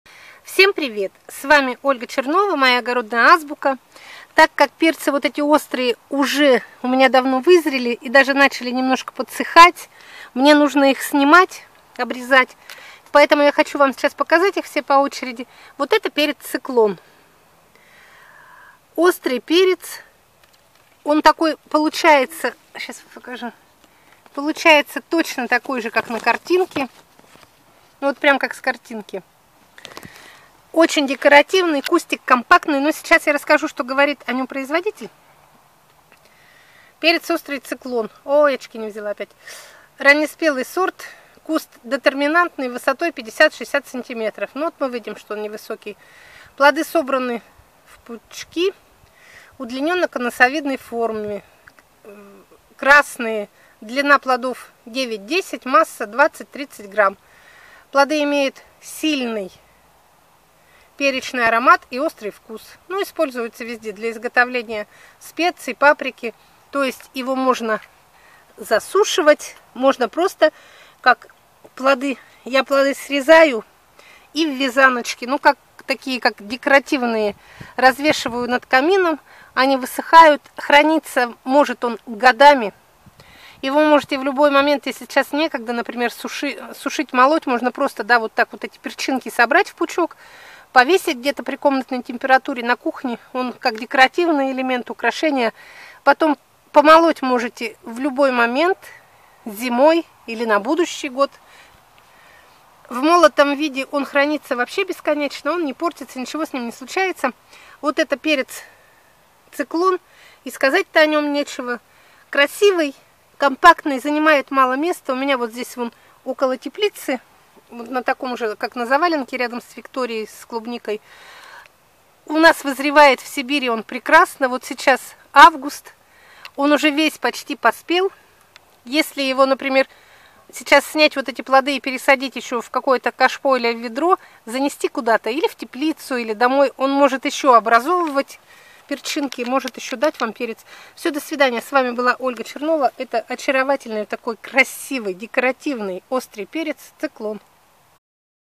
ОСТРЫЙ ПЕРЕЦ ЦИКЛОН - КОМПАКТНЫЙ И ОЧЕНЬ ДЕКОРАТИВНЫЙ. Ольга Чернова.
Красивый, как на картинке! Именно таким вырастает острый перец Циклон - ярко-красные глянцевые плоды поражают своей декоративностью. Кроме практического применения, этот перец может служить украшением участка.